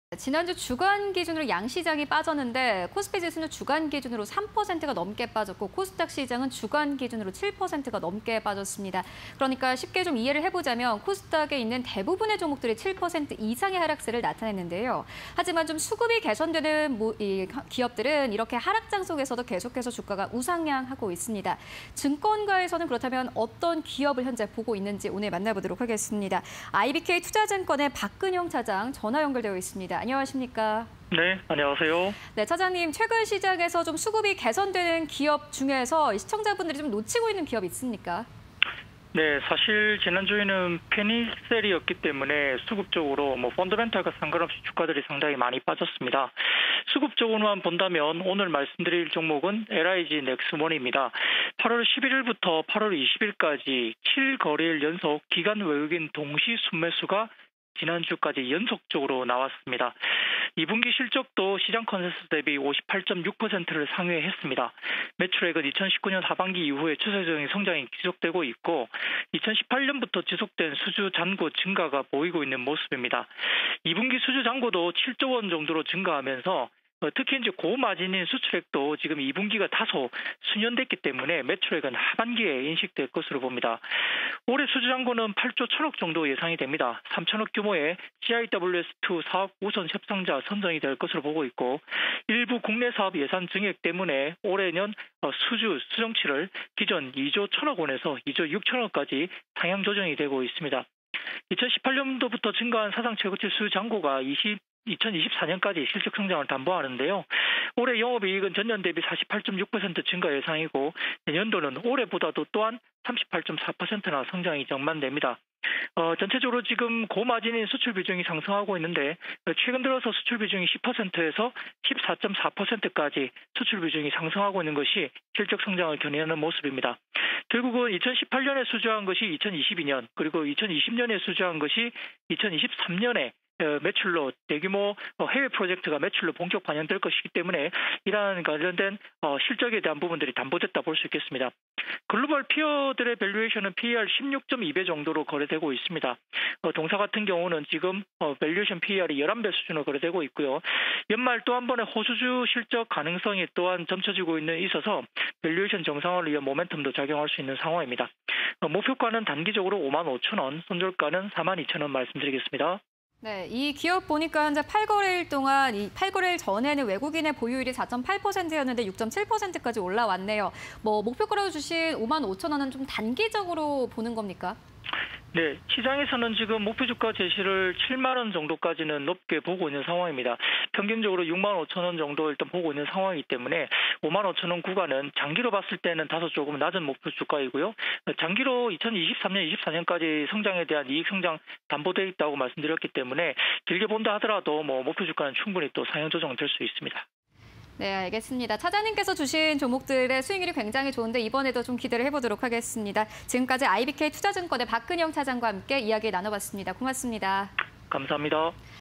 LIG넥스원(079550)... 2분기 영업익 예상치 58.6% 증가_굿머니 공략주 (20210823)
LIG넥스원(079550)
- 2분기 영업익 예상치 58.6% 증가
- 2024년까지 영업이익 성장 확보
- PER 11배… 동종업계 대비 저평가